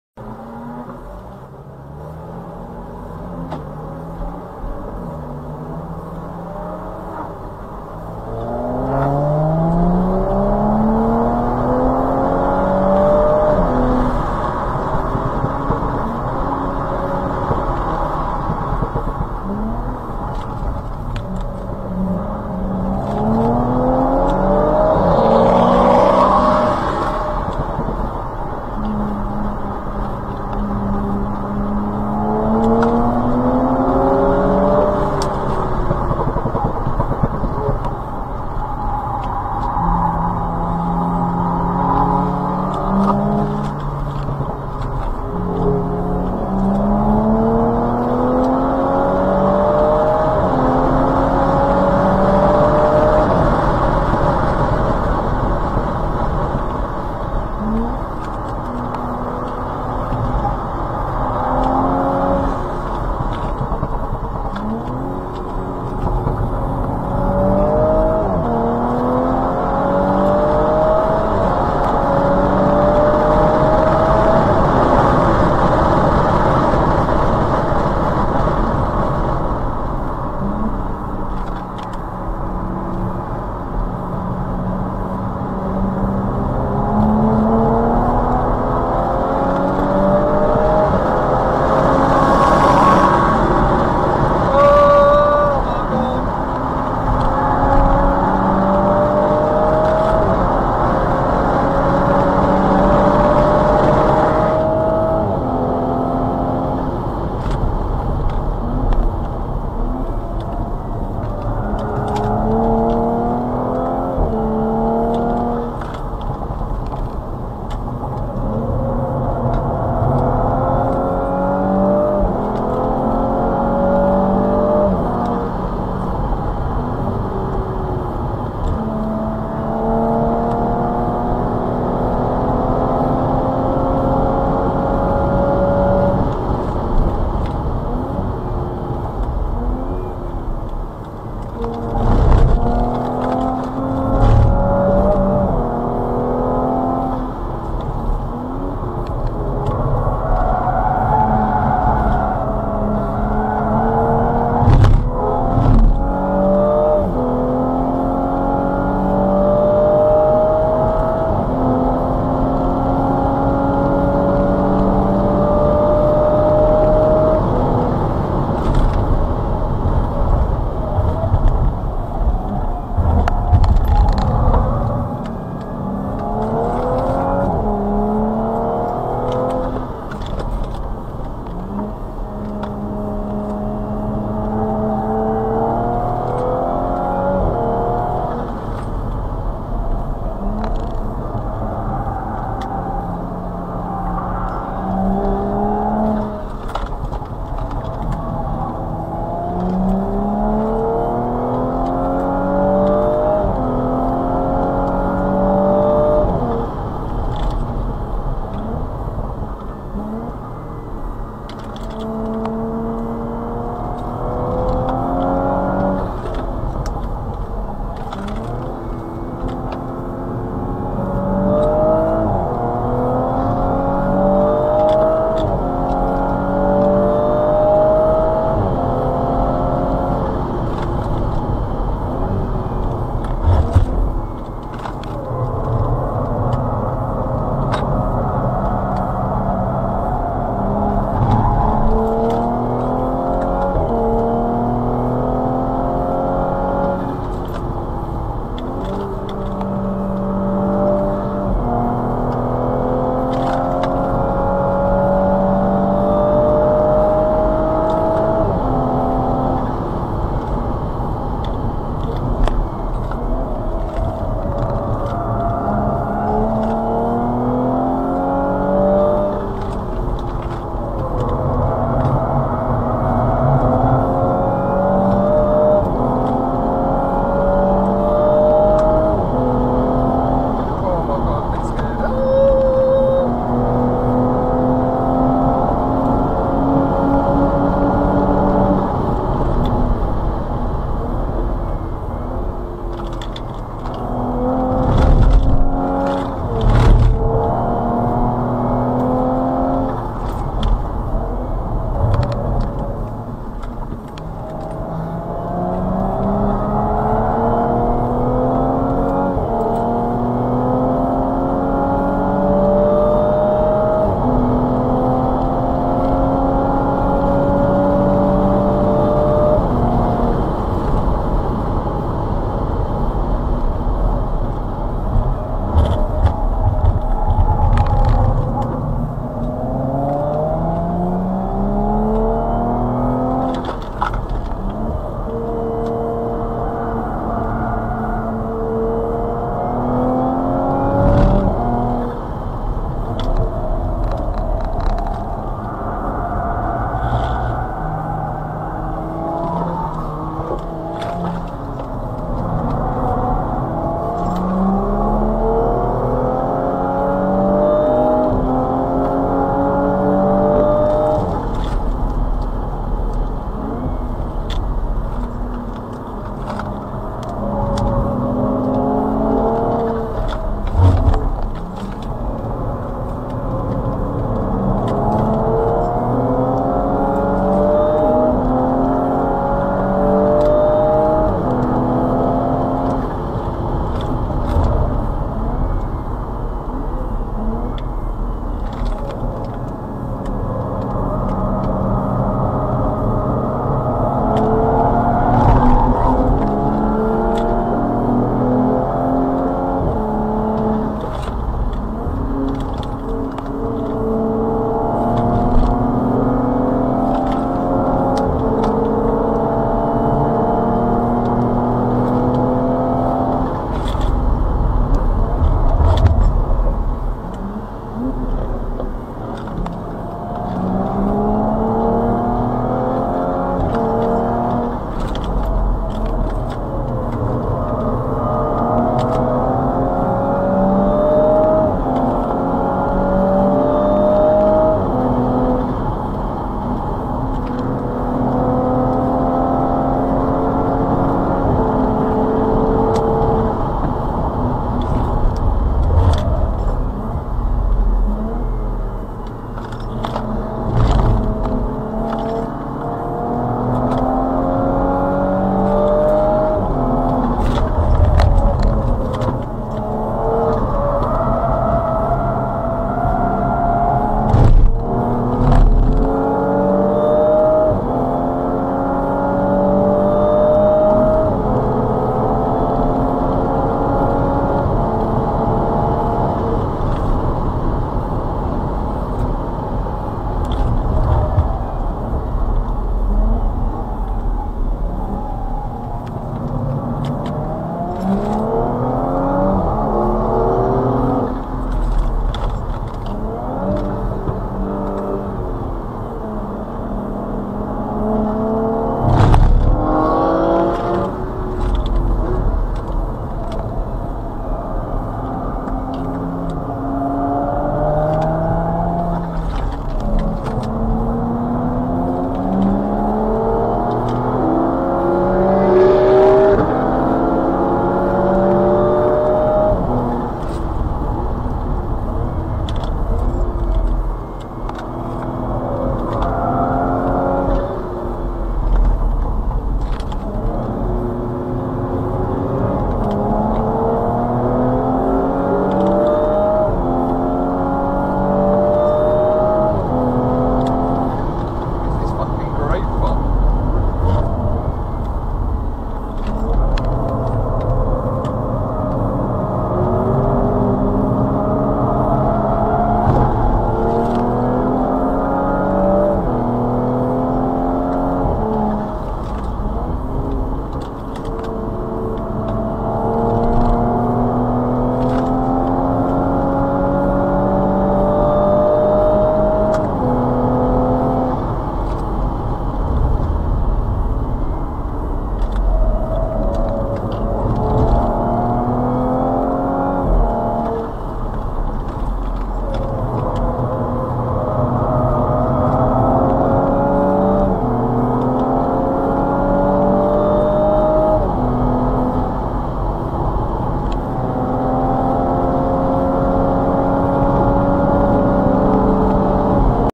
here are some laps of the Grand prix circuit at the nurburgring with our fully stage 2 B9 Audi S4 4
check out what we can do for your car!

here are some laps of the Grand prix circuit at the nurburgring with our fully stage 2 B9 Audi S4 462 BHP and 650 NM!